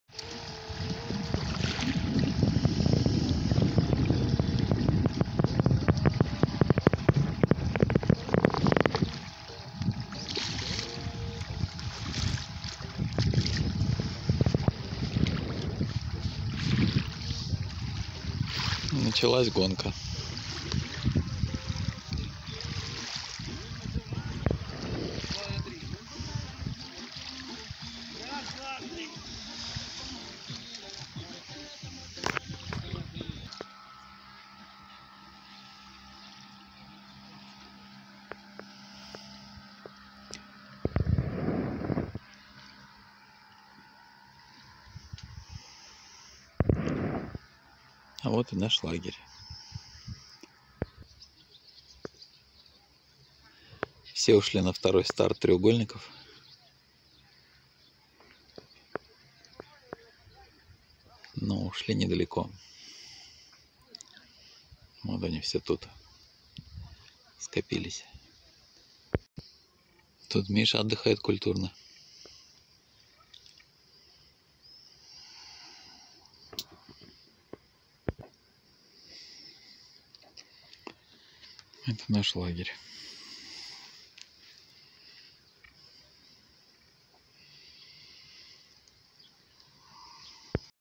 1 мая 2024 г.
Саркел 2024. Парусная регата. 01.05
.2024 Второй старт треугольники. Штиль. Пришел камышовый остров.